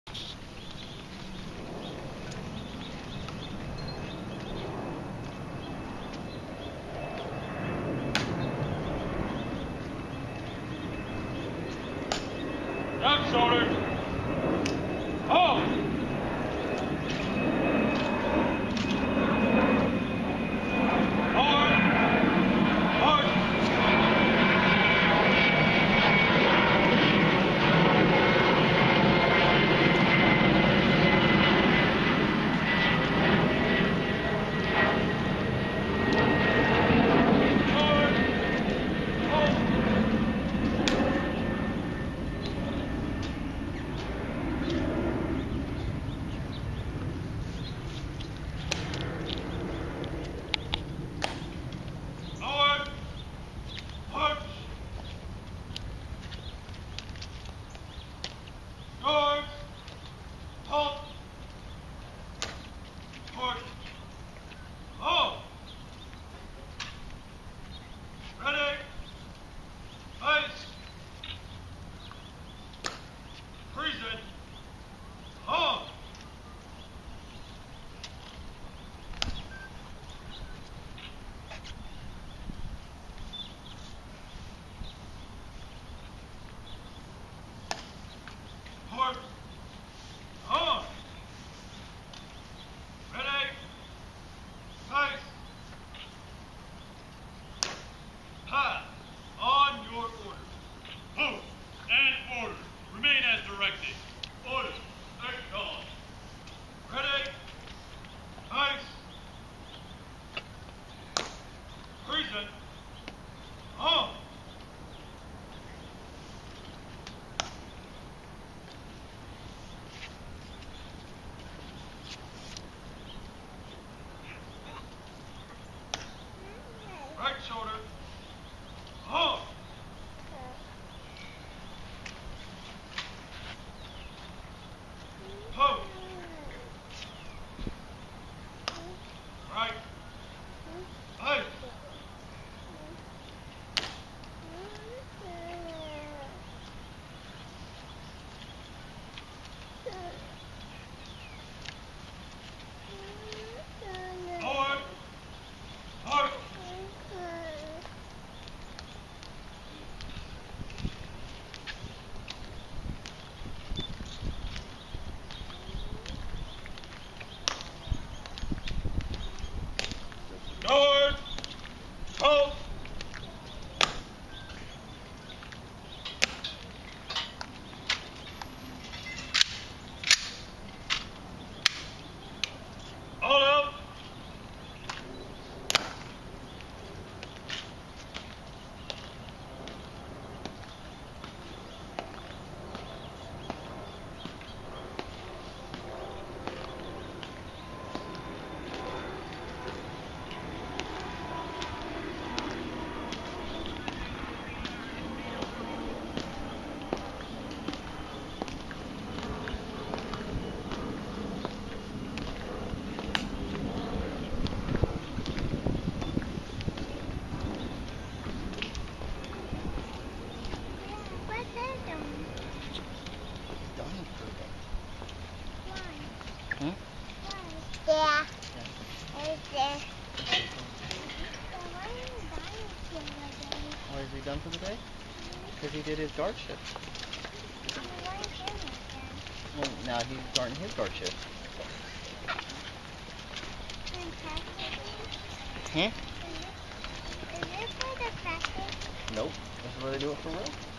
At the Tomb of the Unknown Soldier
My footage of the ongoing watch of the Tomb of the Unknowns at Arlington National Cemetery. As you can probably hear halfway in, it's somewhat of a delight for all ages. Part of The McClain Family Reunion series.